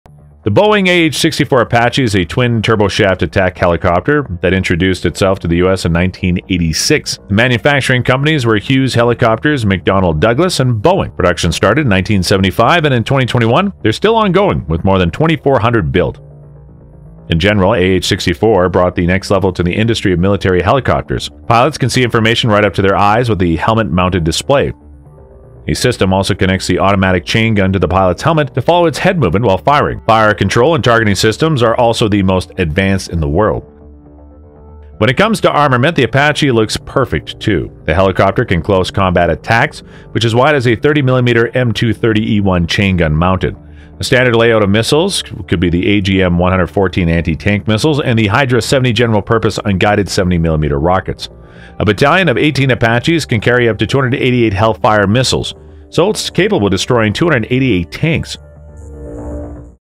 Why the AH-64 Apache is Nightmare To Any Tanks
The Boeing AH-64 Apache is an American twin-turboshaft attack helicopter with a tailwheel-type landing gear arrangement and a tandem cockpit for a crew of two. It features a nose-mounted sensor suite for target acquisition and night vision systems.

The U.S. Army is the primary operator of the AH-64. It has also become the primary attack helicopter of multiple nations.